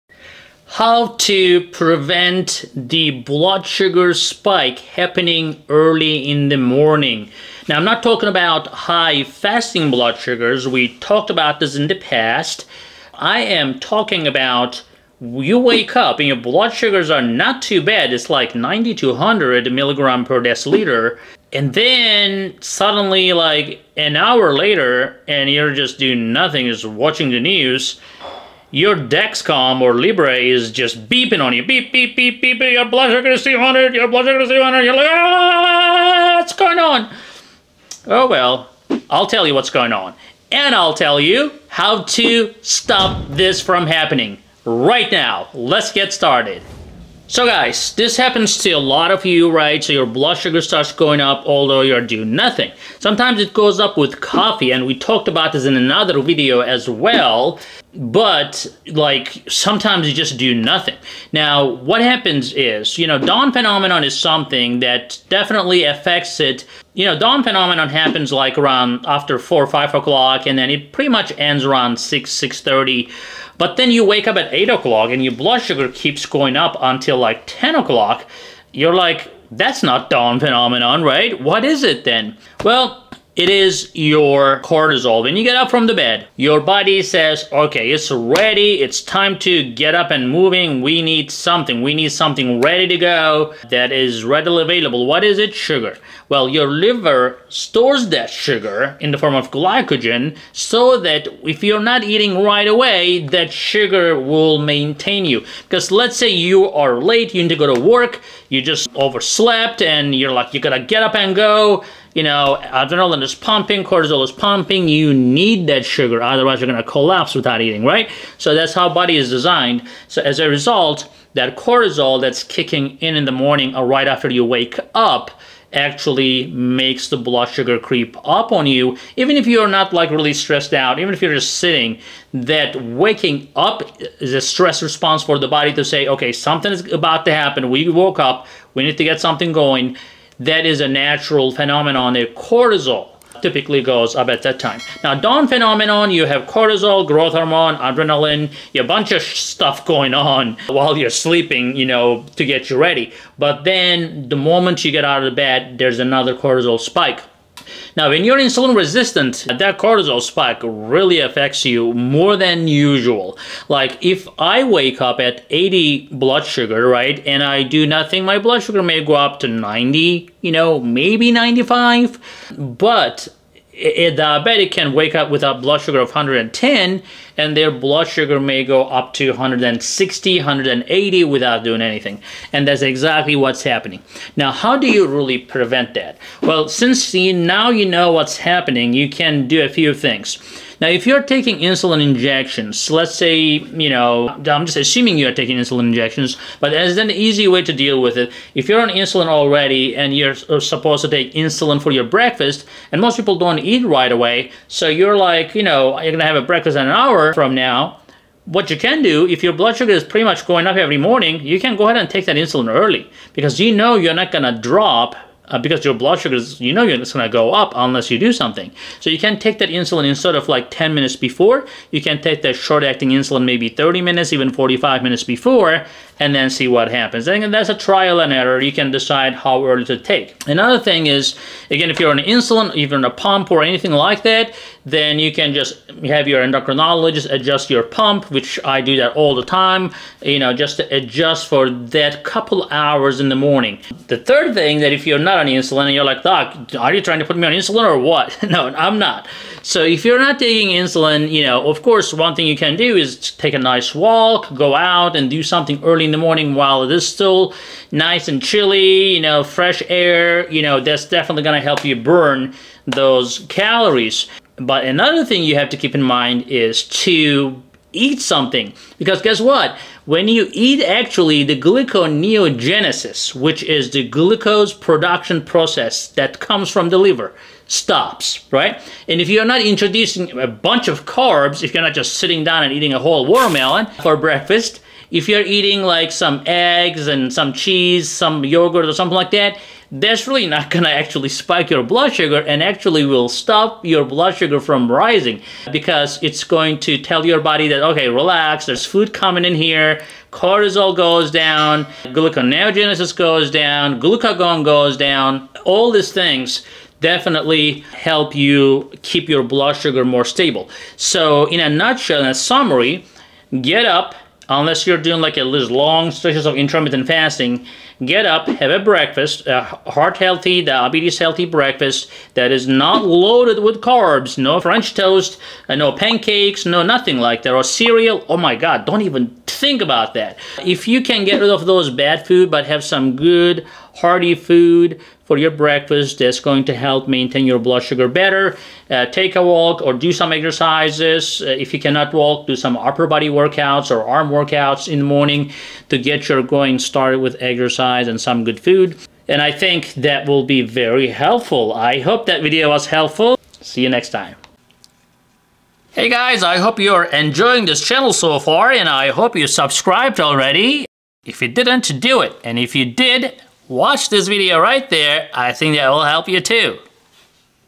Glucose Rising After Waking Up? Here Is Why!
Why does my blood sugar go up after I wake up? Why does the glucose go up before breakfast? Have you ever wondered? Check your blood sugar immediately after waking up and again in 1 hour while still fasting! There are a few reasons for glucose rising after waking up. You will see that blood sugars will be raising even without eating! Yes, Glucose after waking up can be frustrating. Well.. the good news is that it is fixable. Here are the causes of high morning blood sugar after waking up and ways to fix the problem. So lower your blood sugar naturally fast by following these simple steps from SugarMD by Dr. Ergin. Stop the glucose spike in the morning now!